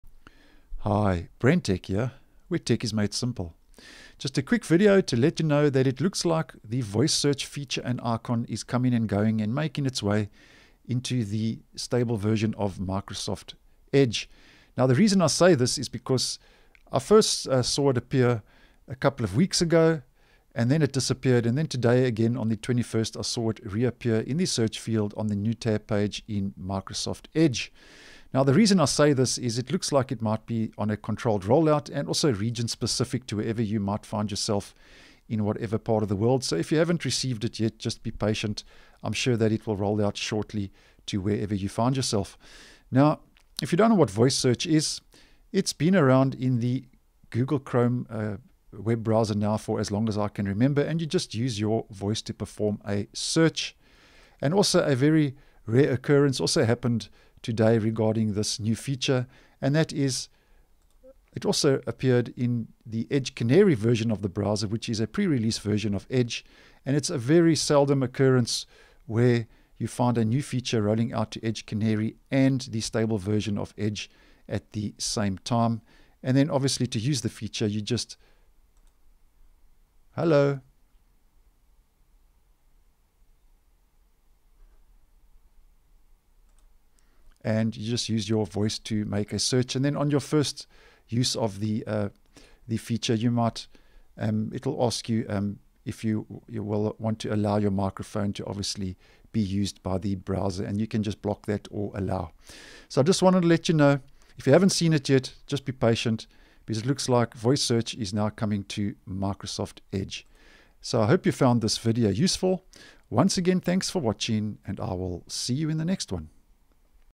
Voice search is becoming available in Microsoft Edge
The feature uses your voice to perform a search.